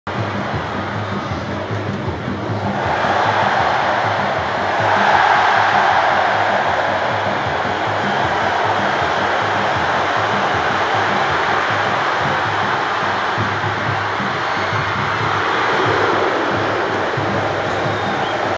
TEM QUE CHAMAR DE NIKÃO GAÚCHO AGORA! HUMILHOU OS CARAS, HEIN?!
O São Paulo perdeu a chance de vencer mais uma no Campeonato Brasileiro 2022, mas teve esse lindo lance do meio-campista no final do jogo. Rapaz... tava frio? Toma esse lençol!

Conheça nossos outros canais:
Canais: